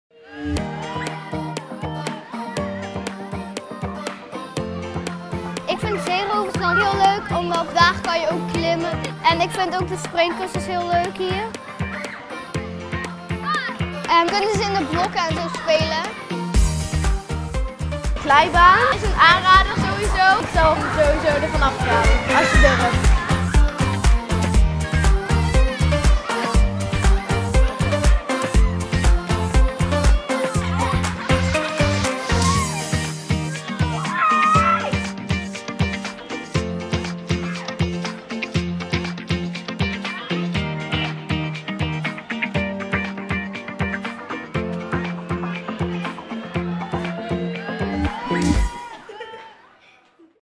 BillyBird Park Hemelrijk | Happy Together | Attractie: Zeeroversland
In deze video maak je kennis met binnenspeeltuin Zeeroversland. Laat je onderdompelen in een piratenwereld en ga samen met je vrienden op avontuur. Voor de kleinere kinderen tot 6 jaar is er een speciale kleuterkade. Hier kun je samen met je kind heerlijk spelen in de verschillende toestellen. Bind een piratendoek om en ga samen op ontdekkingstocht!

Kijk ook op onze Facebookpagina of en blijf op de hoogte van de nieuwste ontwikkelingen bij BillyBird Park Hemelrijk.